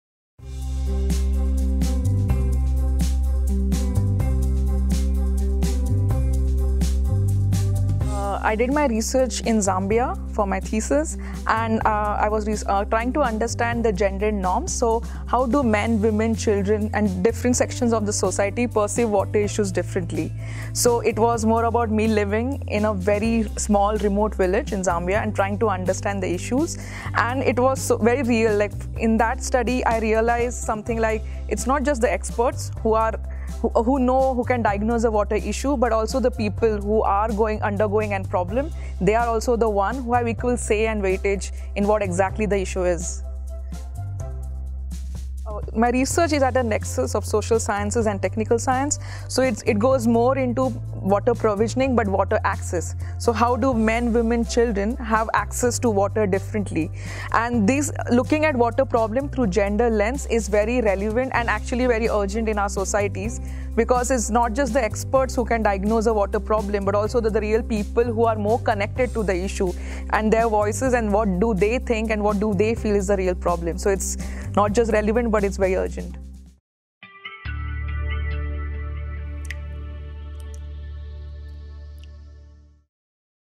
IHE Delft 💧 Interview with MSc student Neha Mungekar
Neha Mungekar comes from Mumbai, India. She wants to apply her knowledge globally. ‘’I feel that water sciences cannot be country specific, but planet specific.’’

MSc Water Management and Governance | Water Services Management | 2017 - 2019